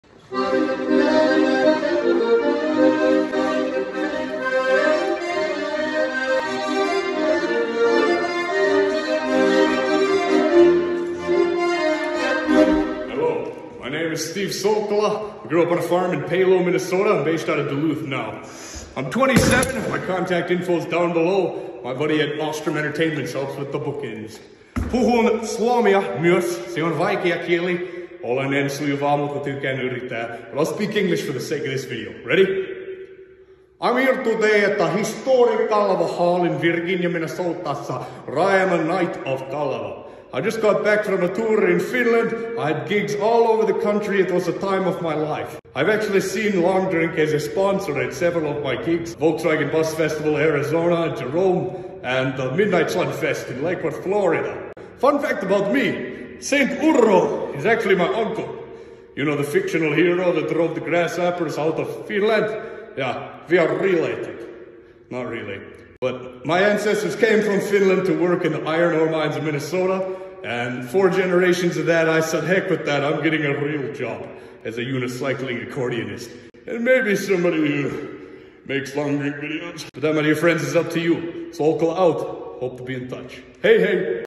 Long Drink Audition Tape - Solkela
There was a casting call sent out to The Finnish and Scandinavian Heritage people looking for a spokesperson for Long Drink!
The audition said to ham it up on the accent 😂
So here ya go! My audition should be good for a few chuckles at least.
If they pick me or not, It was fun to make this clip and use it as content.